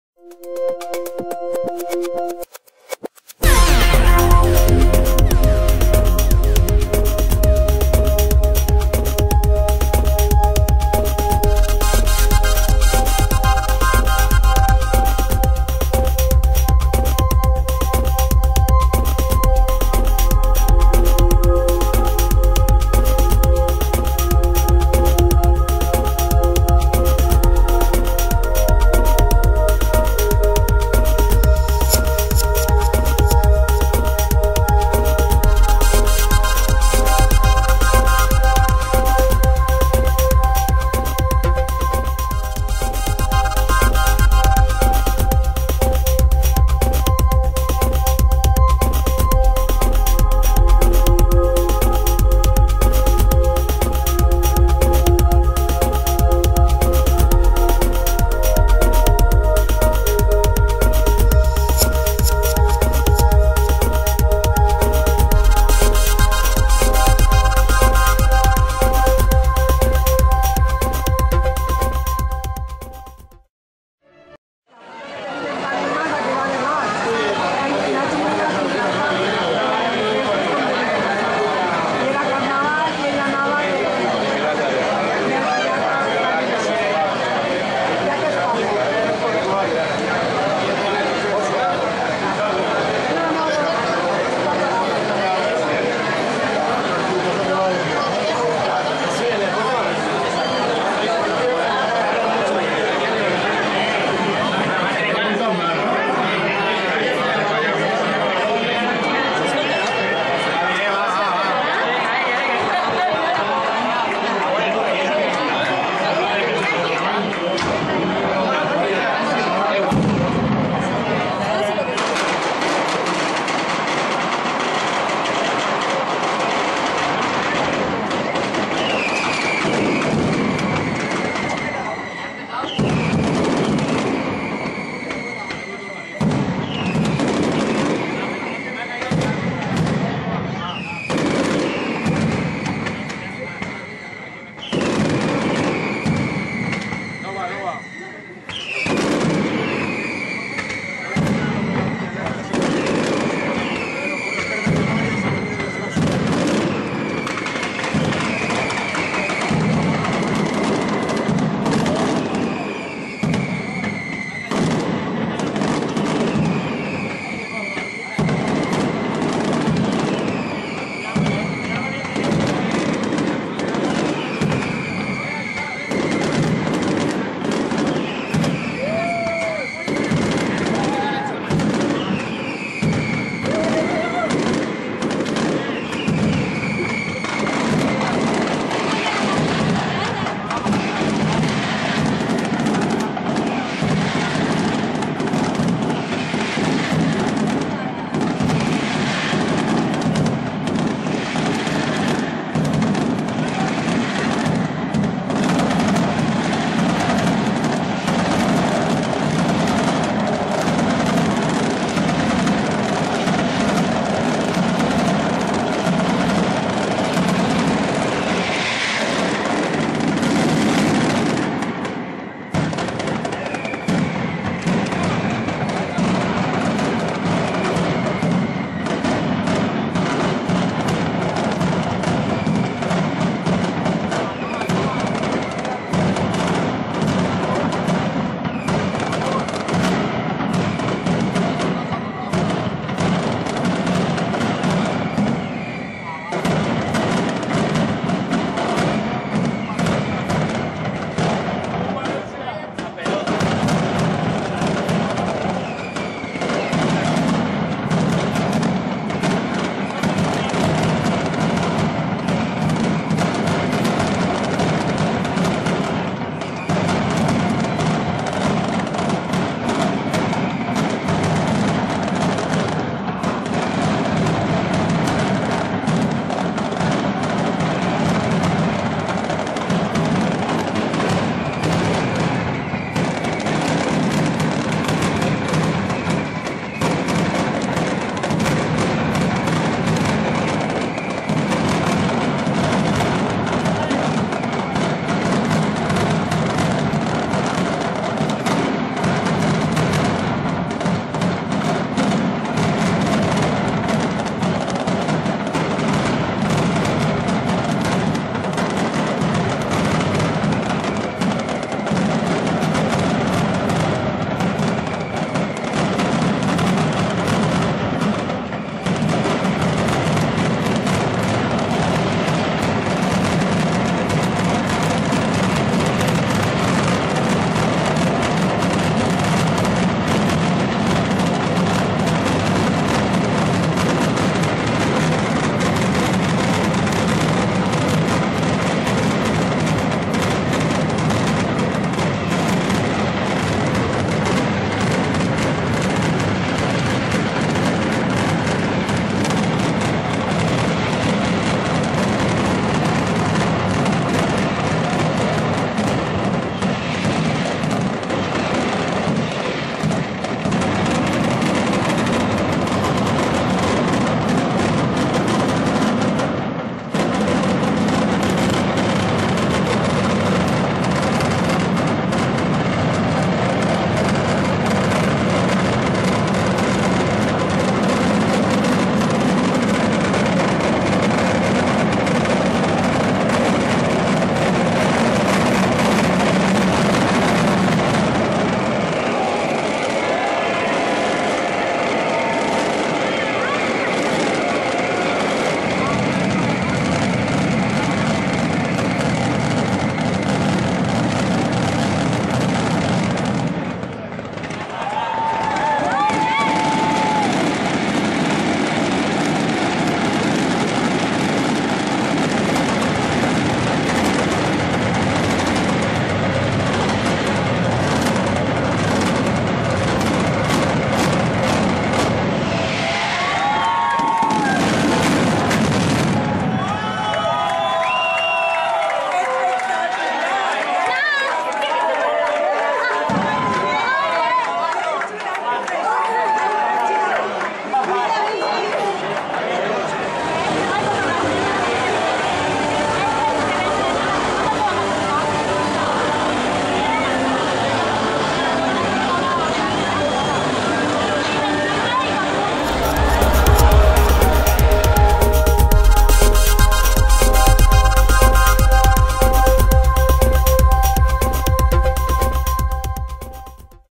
Falles de Valencia 2014 - Mascletà Pirotecnica Elite
Falles de Valencia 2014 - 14 Marzo
Plaza de Ayuntamiento - Mascletà by Pirotecnica Elite

In collaborazione con GUIDA VALENCIA